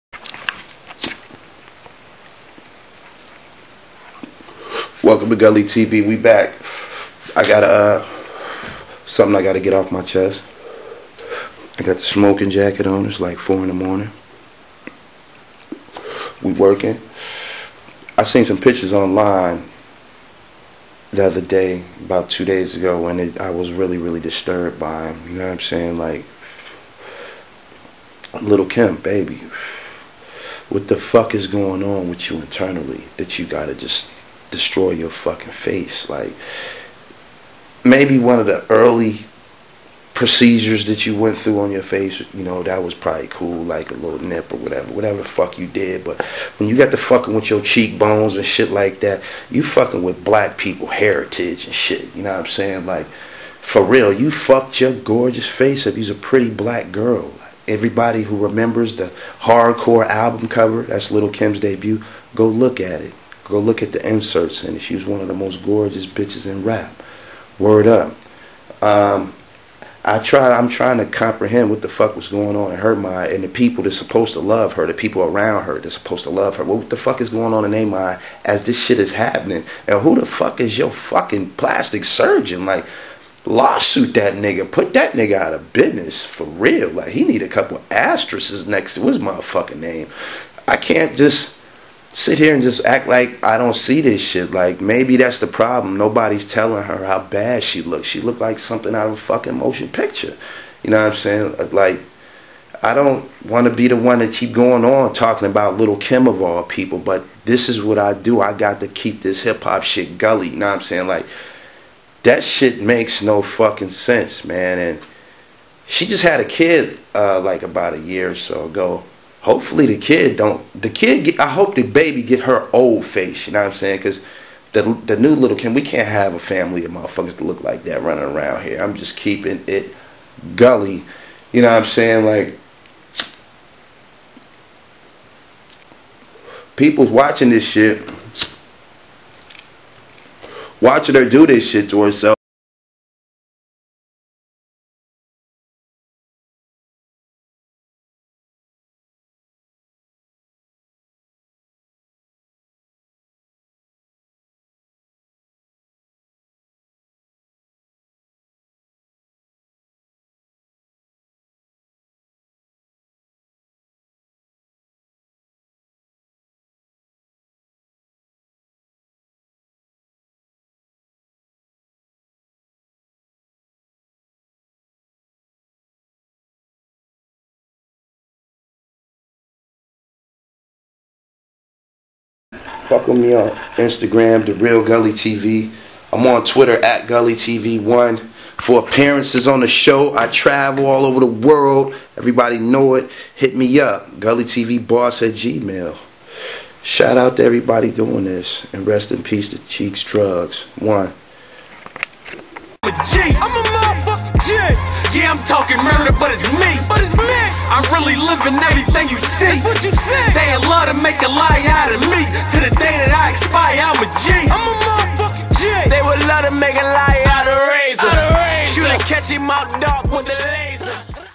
BEFORE & AFTER PICS LIL KIM'S SURGERIES HAS LEFT HER FACE DISFIGURED!!!
GULLY TV TAKES AN UP CLOSE LOOK AT THE DAMAGE DONE TO RAPPER LIL KIM'S ONCE GORGEOUS FACE BY ALL OF THE SURGICAL PROCEDURES SHES HAD OVER THE YEARS